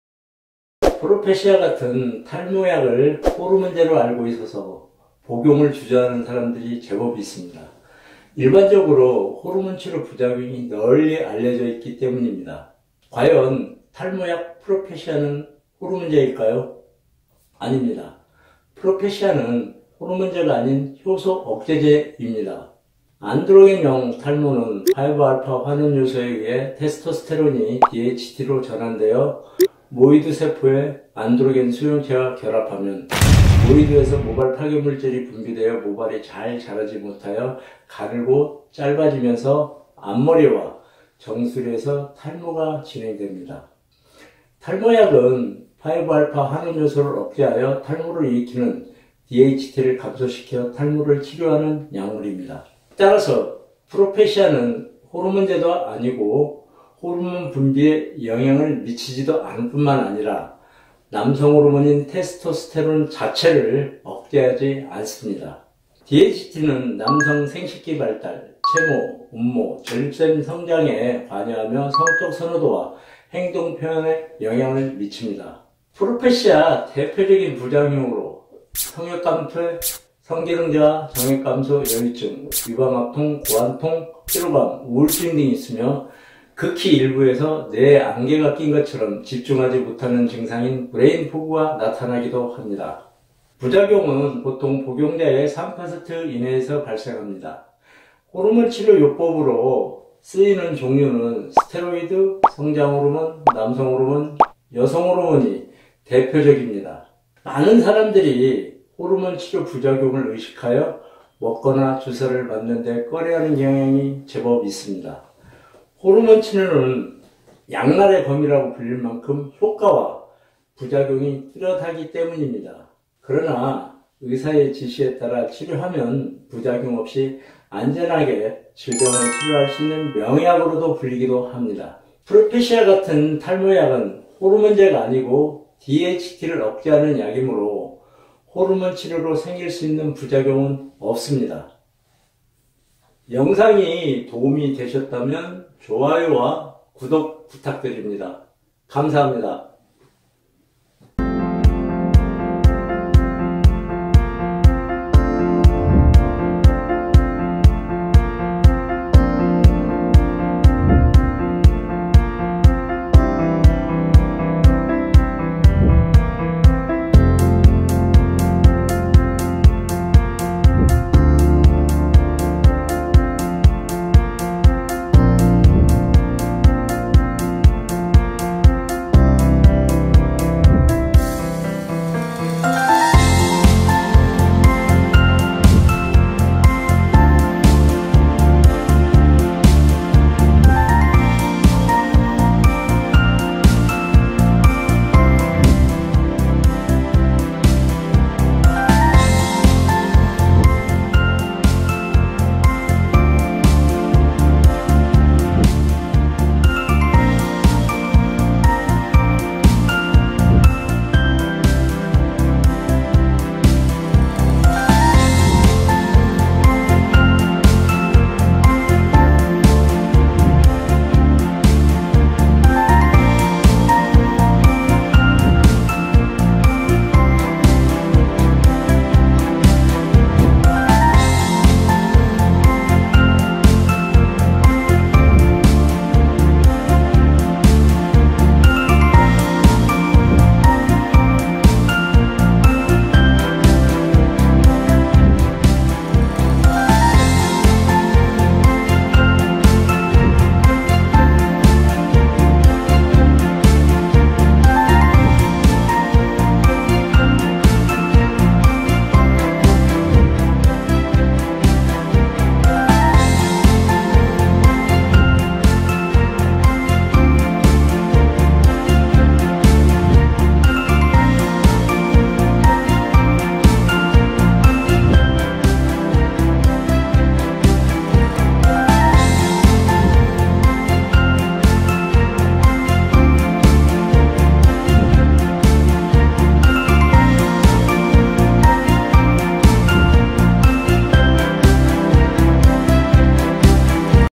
탈모약 프로페시아(성분명 피나스테리드)는 호르몬제인가
피나스테리드 성분의 탈모약인 프로페시아~!

프로페시아를 호르몬제로 알고 있어서 복용을 주저하는 사람들이 제법 있습니다. 왜냐하면 일반적으로 호르몬 치료 부작용이 널리 알려져 있기 때문입니다.

과연 탈모약 프로페시아는 호르몬제일까요?

안드로겐형탈모를  유발하는 주범은 DHT호르몬입니다.
DHT호르몬은 5알파 환원효소에 의해 테스토스테론으로부터 전환된 호르몬입니다.

피나스테리드 성분은 5알파 환원효소를 억제하여 DHT를 감소시키는 작용을 합니다.

즉, 프로페시아는 호르몬제가 아닌 효소억제제입니다.

그럼 동영상을 통해 자세히 알아볼까요?

✅홍성재 원장이 직접 운영하는 탈모블로그